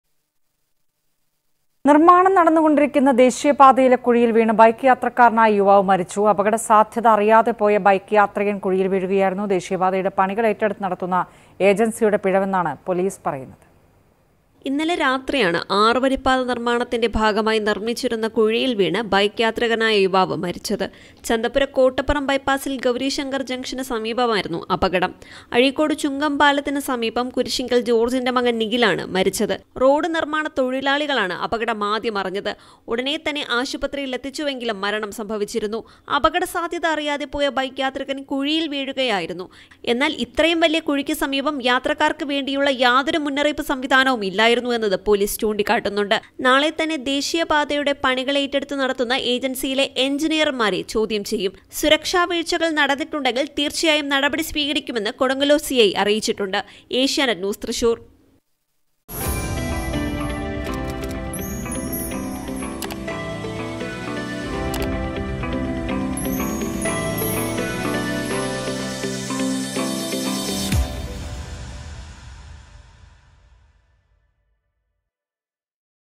മുന്നറിയിപ്പ് ബോർഡ് വെച്ചില്ല, റോഡിലെ കുഴിയിൽ വീണ് ബൈക്ക് യാത്രികന് ദാരുണാന്ത്യം | Thrissur | Road
റോഡിലെ കുഴിയിൽ വീണ് ബൈക്ക് യാത്രികന് ദാരുണാന്ത്യം; ബൈക്ക് വീണത് ദേശീയതപാത നിർമ്മാണത്തിനായി കുഴിച്ച കുഴിയിൽ;  മുന്നറിയിപ്പ് ബോർഡില്ലാത്തതിൽ കരാർ ഏജൻസിക്കെതിരെ പരാതി